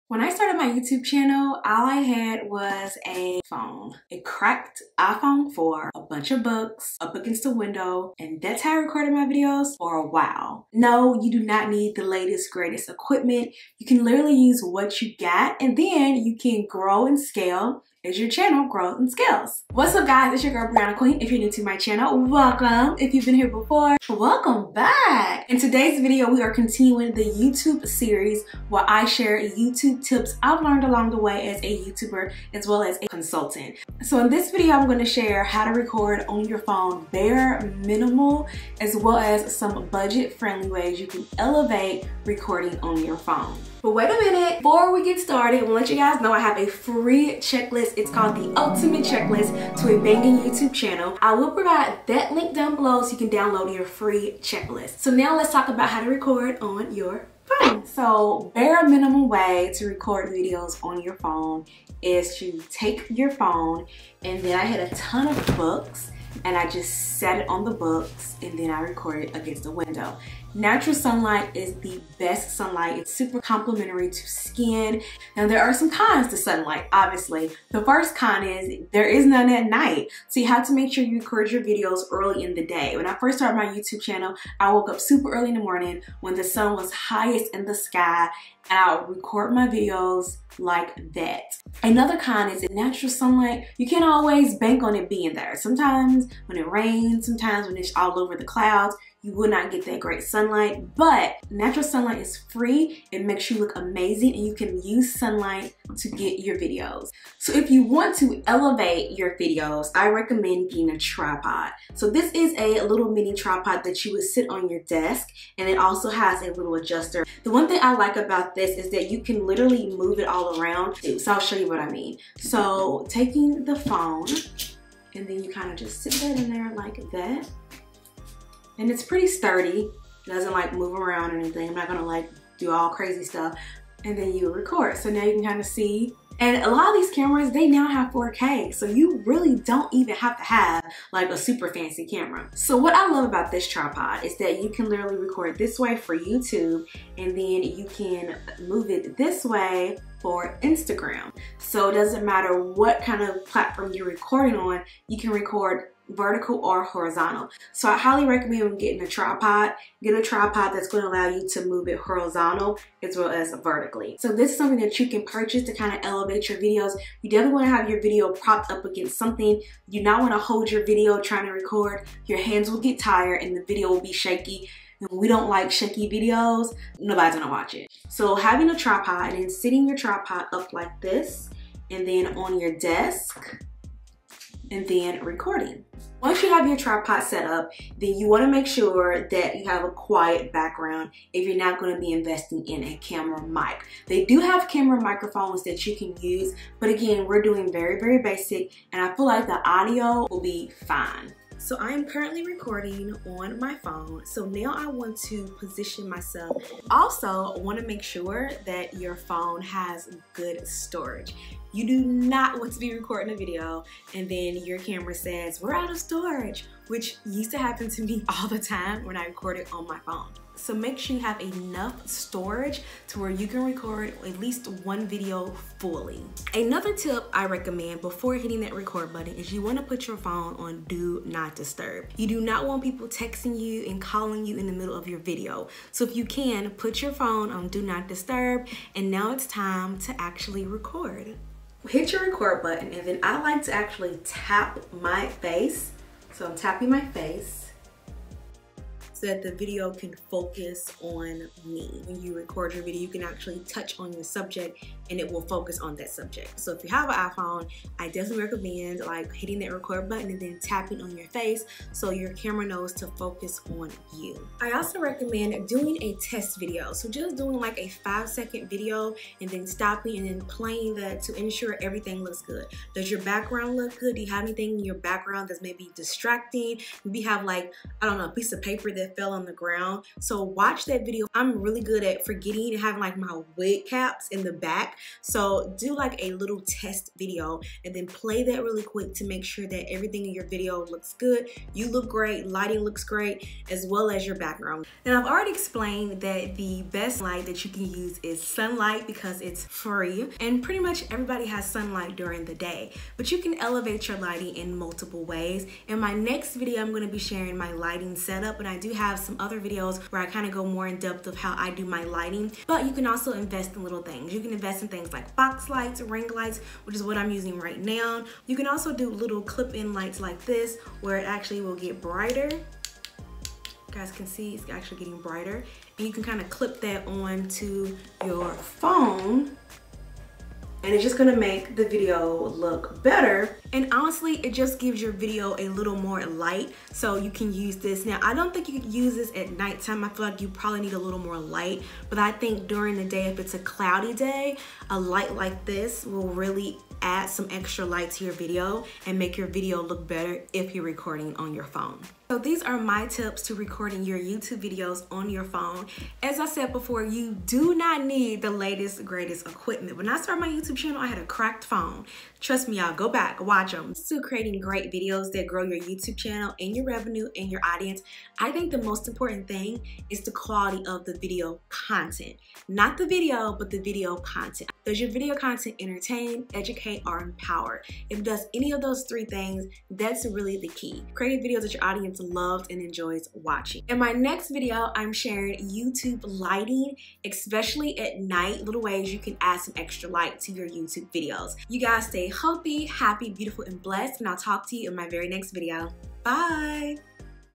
How I RECORD my videos on my IPHONE (YouTube Tips series)
We are continuing the YouTube series where I share tips to start and grow a successful YouTube channel as a beginner! In this video, I am sharing how to record a youtube video on your phone. I am sharing affordable ways to record a high-quality video on your phone and ways to make your youtube video look better recording on an iphone.

❤️ PIN to your Pinterest board so you can reference this video later!
❤️ Share with any friends who want to start or grow their new YouTube channel!

♥ Let’s be friends on other platforms! ♥

CHAPTERS:

Intro 0:00
Position your phone 1:00
Tripod Tip 1:50
Check you sound (quiet background) 3:10
Ensure you have decent storage 3:30
Put your phone on dnd 3:50
Have your Focus on you 4:03
Do a test video to ensure everything is great 4:24

Tips for lighting 5:05

TOOLS USED & CALL-OUTS:
Phone Light